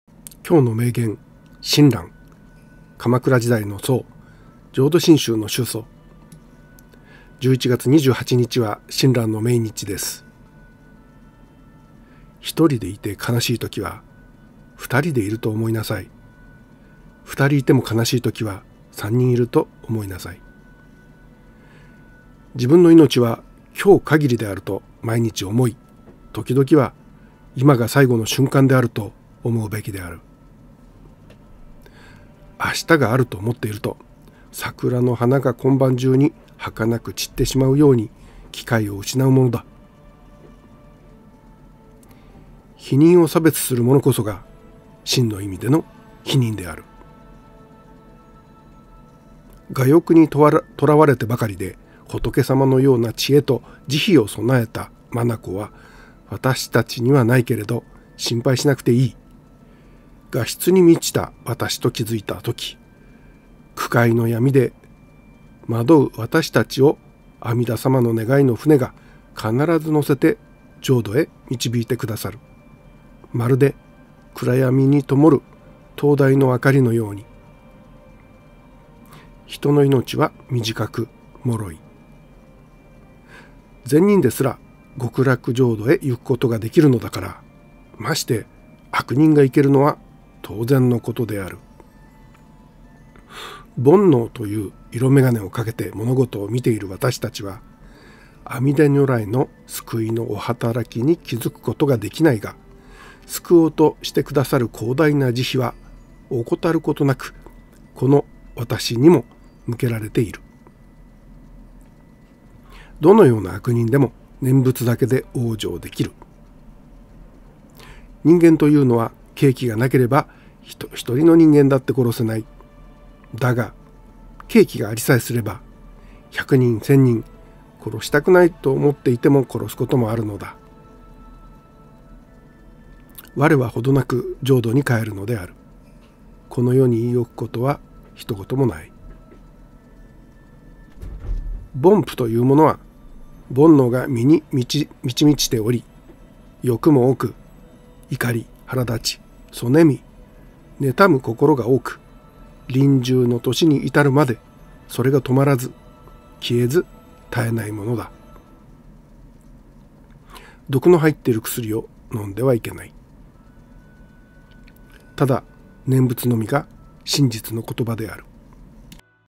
【今日の名言 11月28日】親鸞（浄土真宗の宗祖）
「一人でいて悲しいときは二人でいると思いなさい｡二人いても悲しいときは､三人いると思いなさい」
【今日の名言 11月28日】親鸞（鎌倉時代の僧､浄土真宗の宗祖）